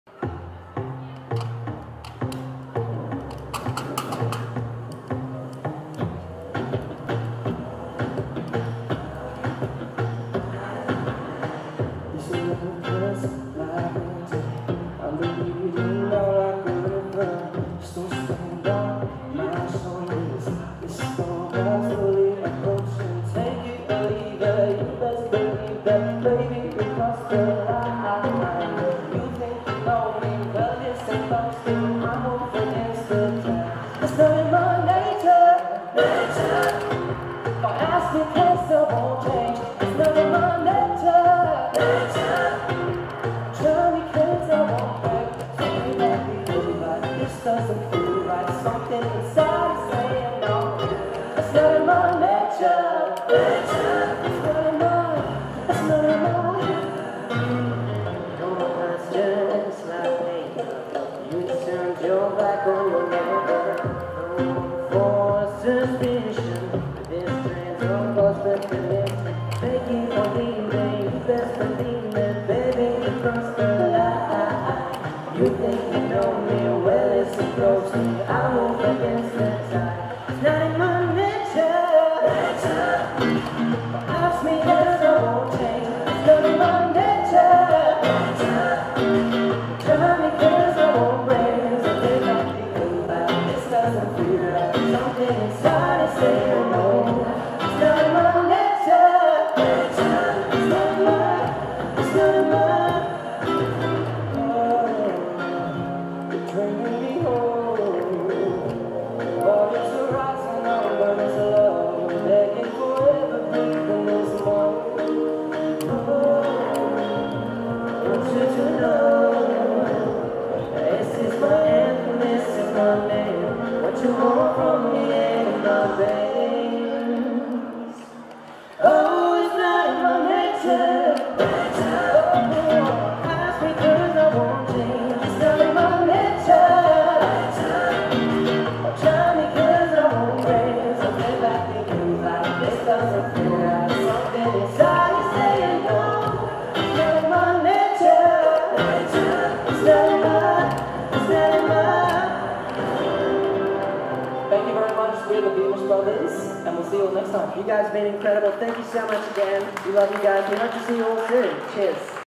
Brisbane City Sounds Thea Beamish Brothers Nature
The Beamish Boys are two musical brothers whose passion for singing and songwriting started in their garage and has landed them in front of international audiences. Based in Toowoomba QLD, Jeremy (18) and Ben (20) combine an authentic blend of two brothers harmonising with fresh songwriting ideas to hone in on their signature acoustic style. With an original EP and four follow-up singles under their belt, 2017 saw Ben and Jeremy take their music on a three-month tour of Spain and the US, crafting their live shows in impromptu street concerts and sold-out gigs. All the while, The Beamish Boys have channelled their experiences into new exciting music.

Photo credit: @ibismusicphotography